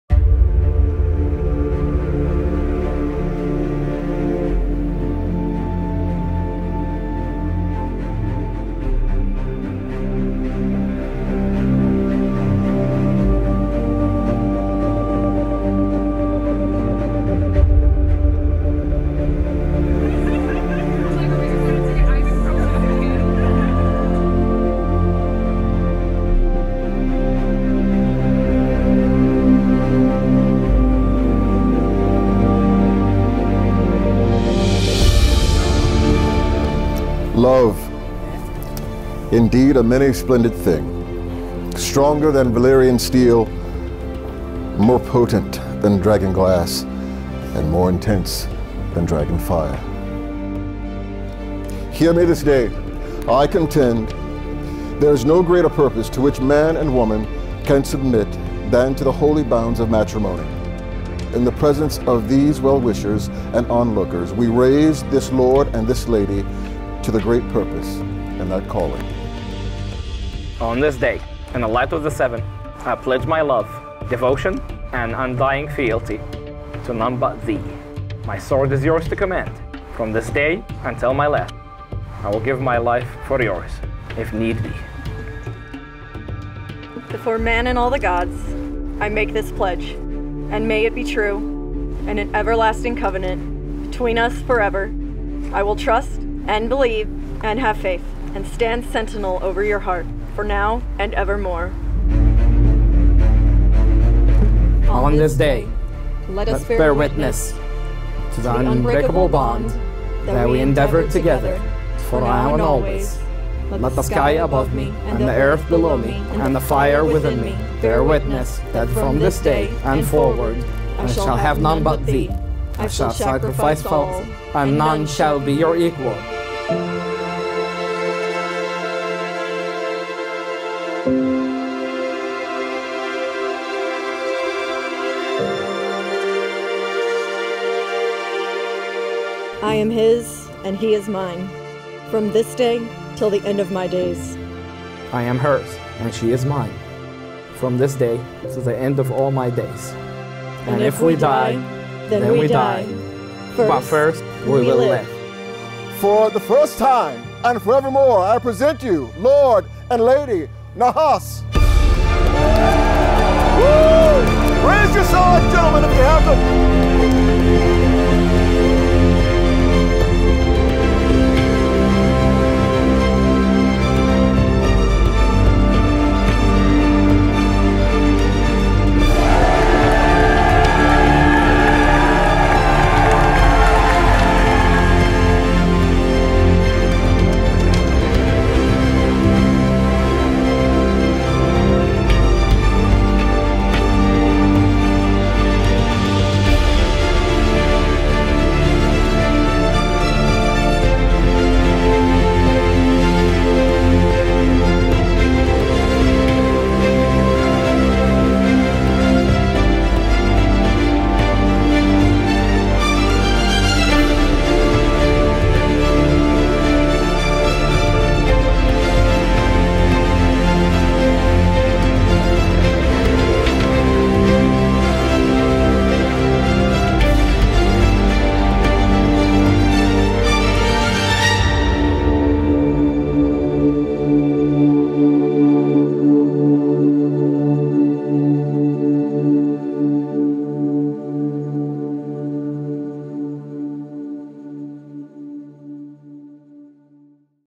Sandy and Omar's Game of Thrones/Skyrim Themed Wedding Highlight Video
If you miss Game of Thrones and you want a little taste of the glory, check out Sandy and Omar's Game of Thrones/Skyrim inspired wedding! This was a one of a kind experience and we had so much fun filming and editing it.

Planner - Cristy Leeds
DJ - Cameron Hoeben